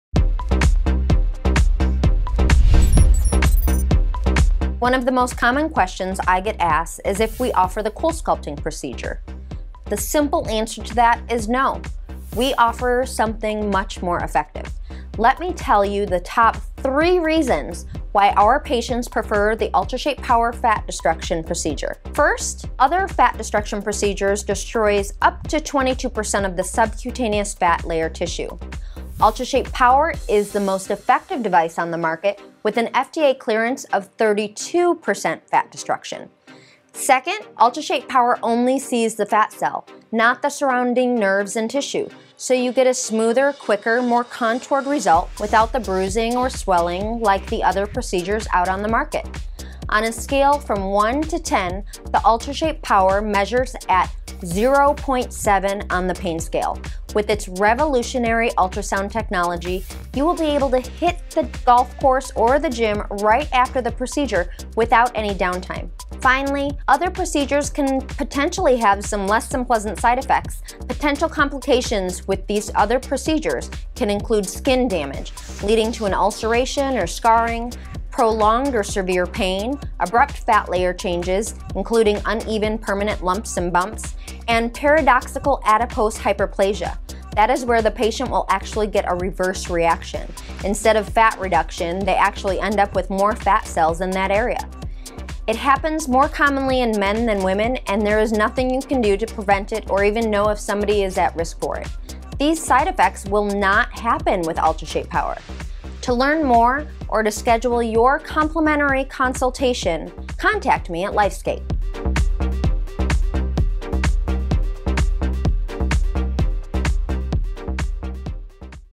Permanent Fat Reduction with UltraShape Power - LifeScape Aesthetics
LifeScape Premier's medical aesthetitican, Noelle Cesario, explains the sculpting potential of UltraShape Power (stronger, safer, and less downtime than cool sculpting methods).